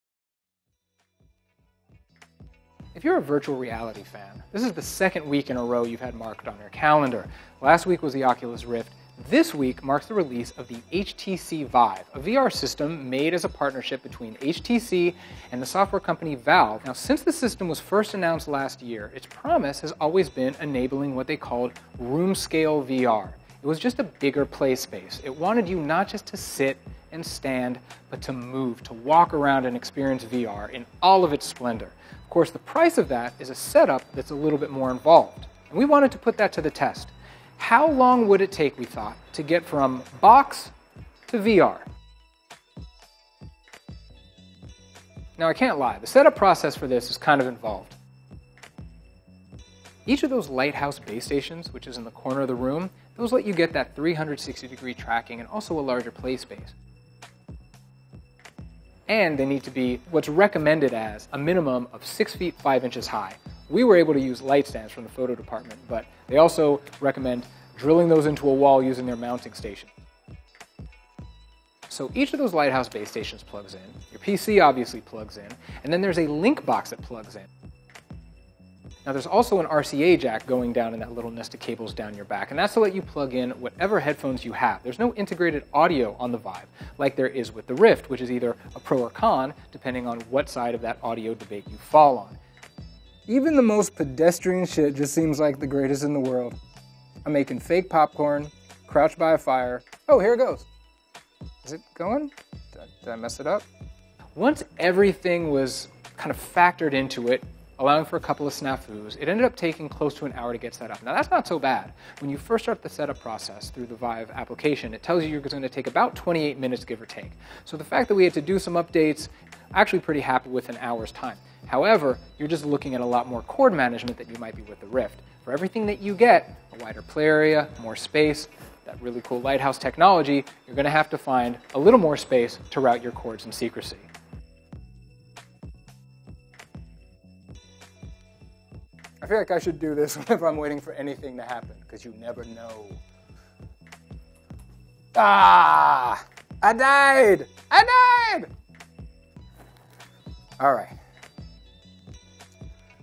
Setting Up an HTC Vive: Plug and Plug and Plug...Then Play!
The HTC Vive system is meant to deliver what HTC and Valve call “room-size experiences”— that means you’ll need an entire room, and a good chunk of time, to get  your VR playpen set up.

Watch This Timelapse Video of Us Setting Up an HTC Vive

Editor: Nurie Mohamed